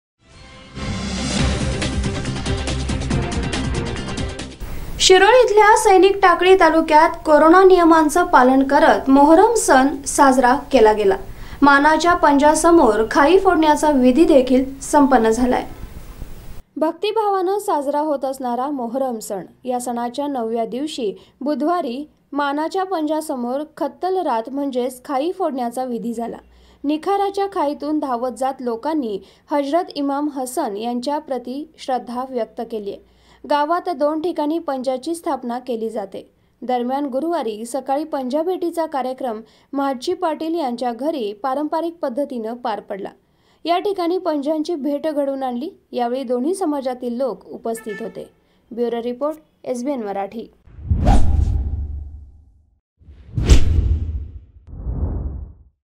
Kolhapur | शिरोळ सैनिक टाकळी तालुका येथे कोरोना नियमांचे पालन करत मोहरम सण साजरा,
शिरोळ सैनिक टाकळी तालुका येथे कोरोना नियमांचे पालन करत मोहरम सण साजरा, मानाच्या पंजासमोर खाई फोडण्याचा विधी संपन्न.@